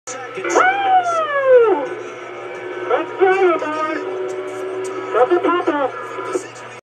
Greg Biffle does a Ric Flair impression during Michigan race Woooo!
Via NASCAR Hotpass on DirecTV

#16 Greg Biffle who replaced #2 Kurt Busch on one of the feed after his engine blew up, is very happy at how quick he is going and does a Ric Flair Wooooo!